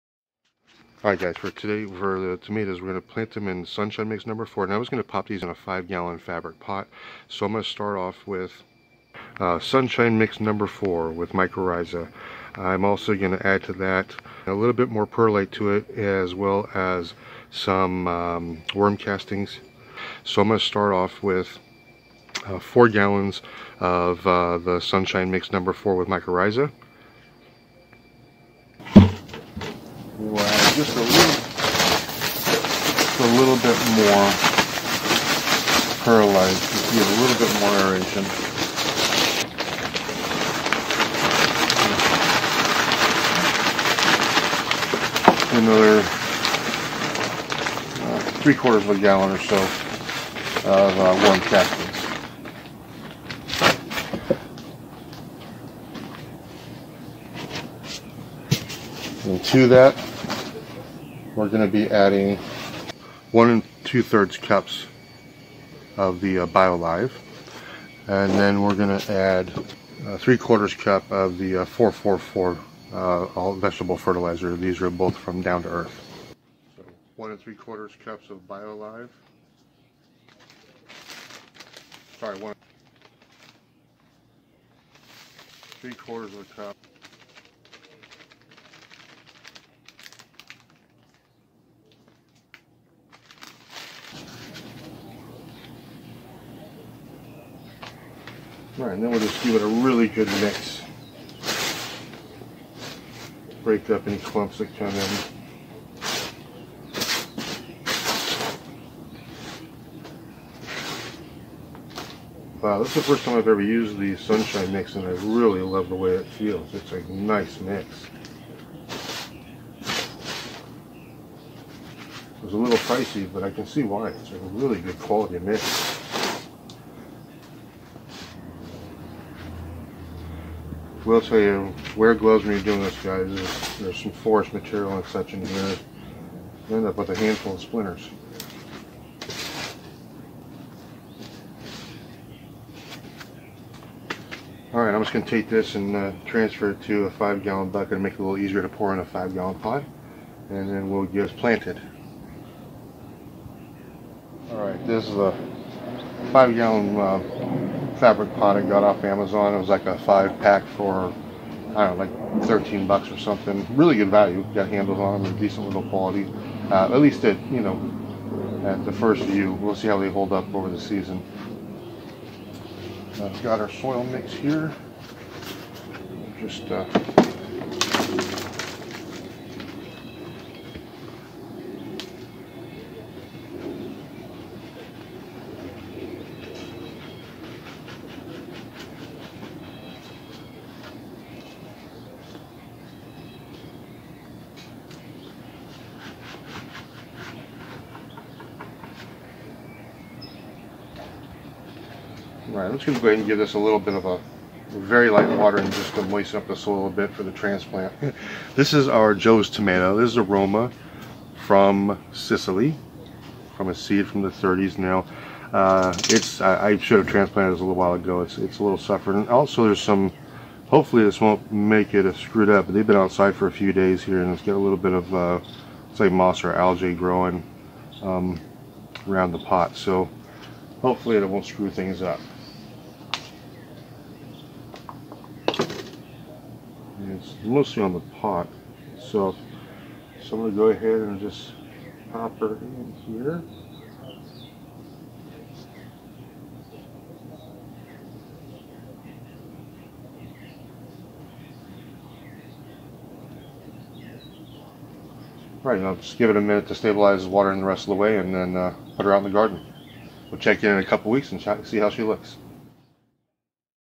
Heritage Roma Tomato II Building the soil and transplanting
Today I'm building an organic soil for my Joe's Tomato and transplant the seedling.
This plant is from a seed that was brought from Sicily in the 1930's and has been handed down family to family since. Follow along as we grow this delicious Roma tomato.